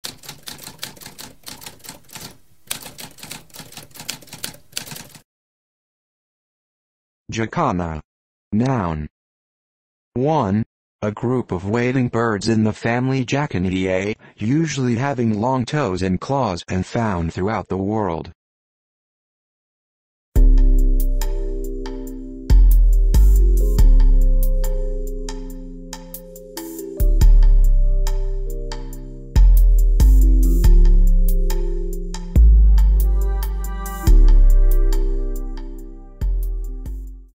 What does jacana mean?
A spoken definition of jacana.

Intro Sound:
Typewriter - Tamskp